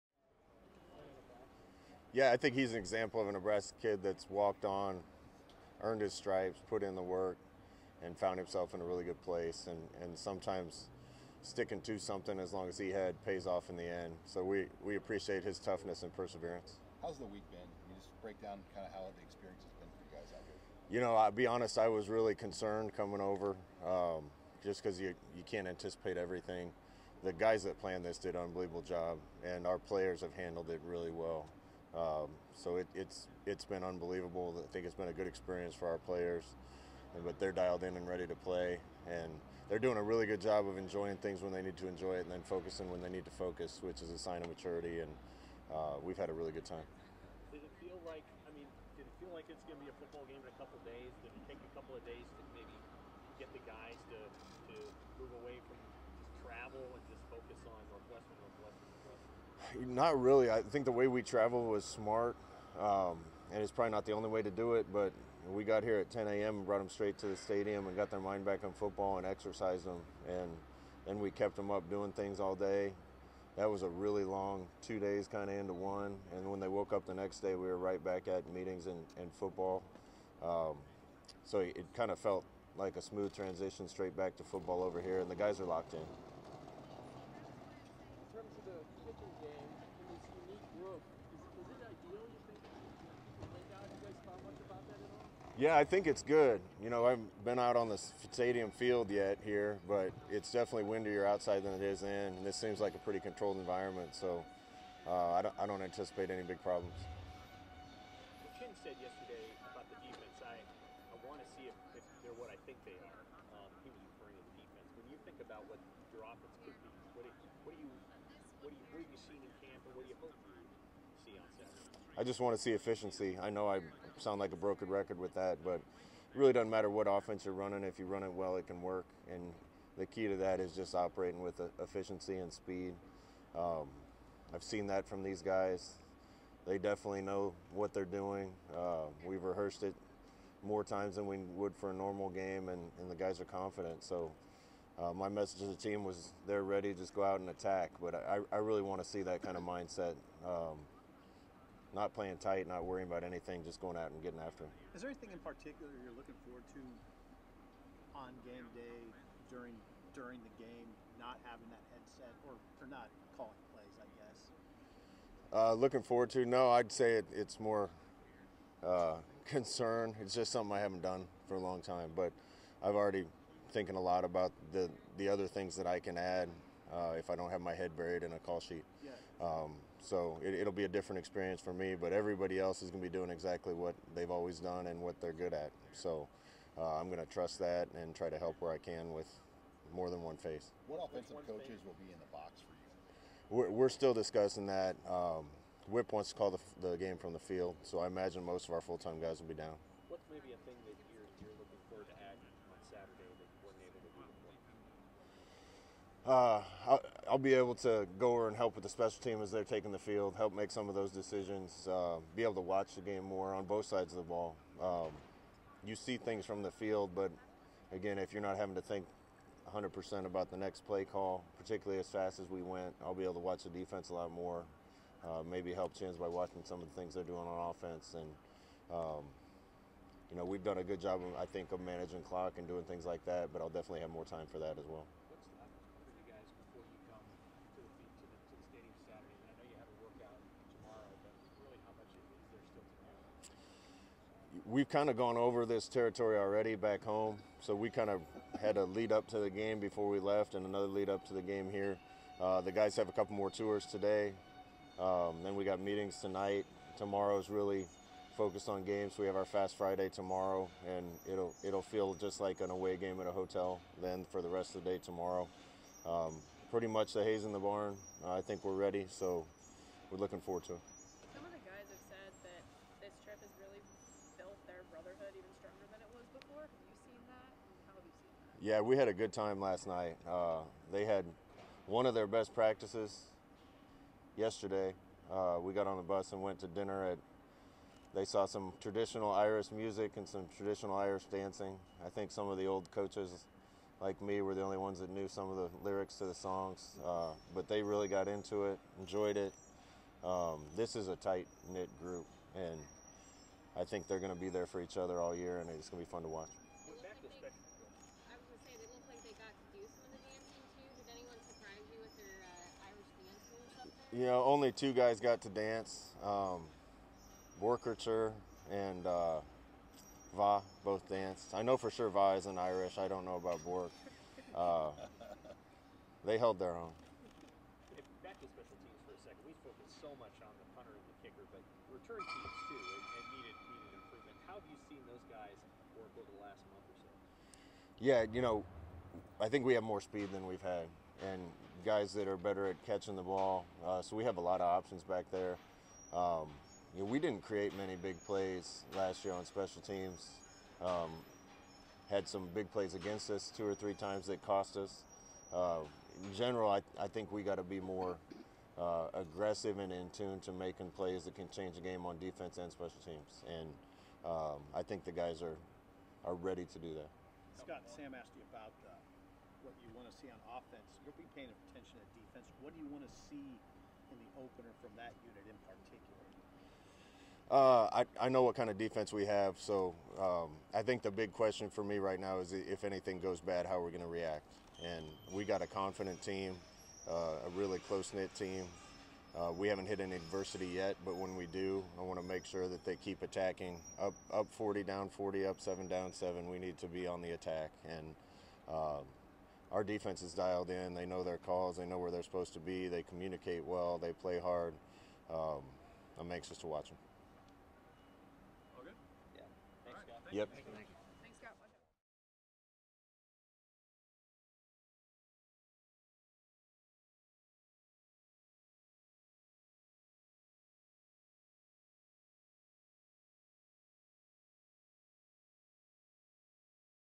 Nebraska Football: Scott Frost Talks Game Prep and More
The Nebraska football team practiced outside of Aviva Stadium on Thursday in Dublin, Ireland. Coach Scott Frost met with the media afterward to discuss the Huskers' game preparation, what he hopes to see on Saturday and much more.

Presented by Union Bank & Trust